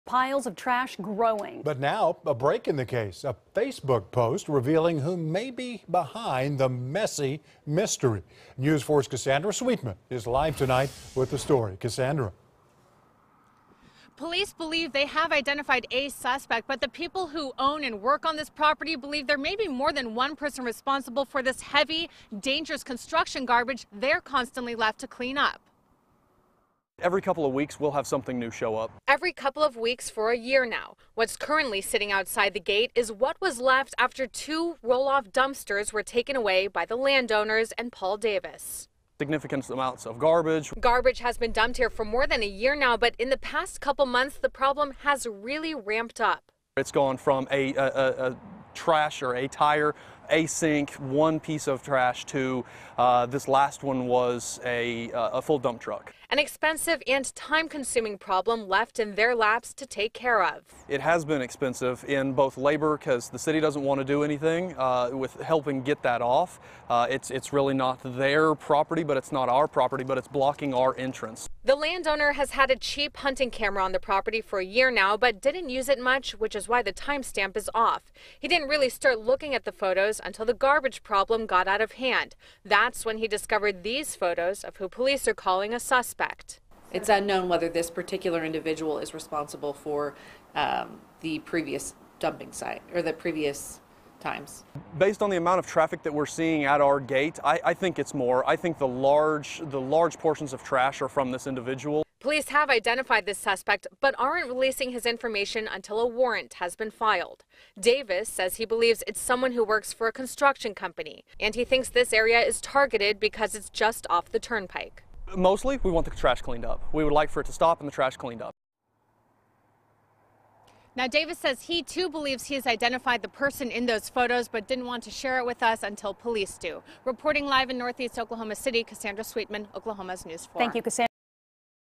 Suspect identified after year of garbage left at metro stranger`s gate
After a year of having growing piles of garbage dumped on their property, a metro man and his friend have a good idea of who is responsible, and so do police.